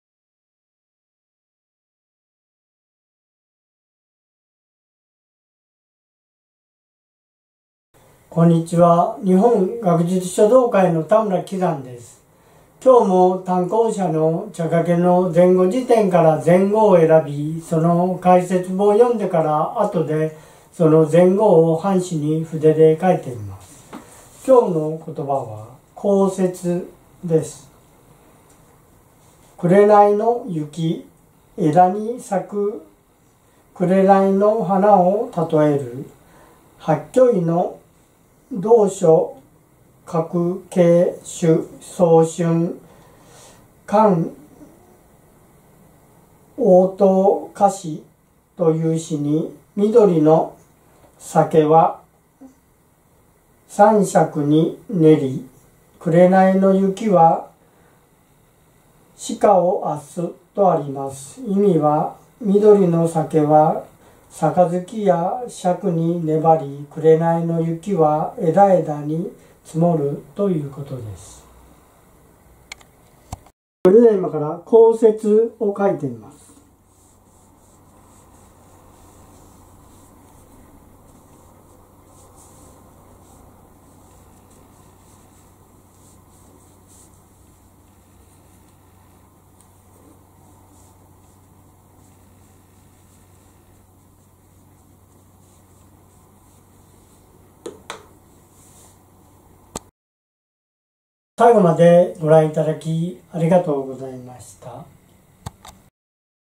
田村季山先生による禅語解説と範書「紅雪」
本日は「紅雪（こうせつ）」です。